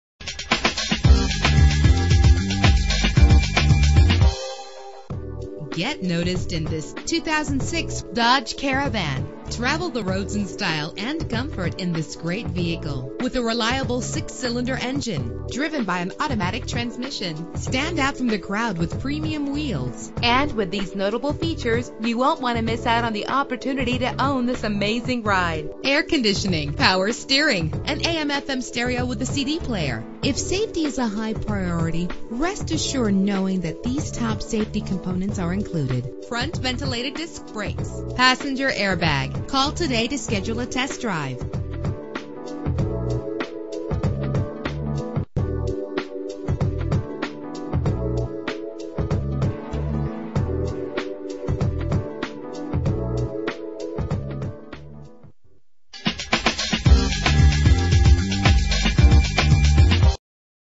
2006 Dodge Caravan - CHESHIRE MA
Year: 2006
Make: Dodge
Model: Caravan
Trim: SE
Engine: 3.3 liter 6 cylinder 12 valve
Transmission: 4-Speed Automatic
Color: Blue
Mileage: 70016
Address:
452 S State Rd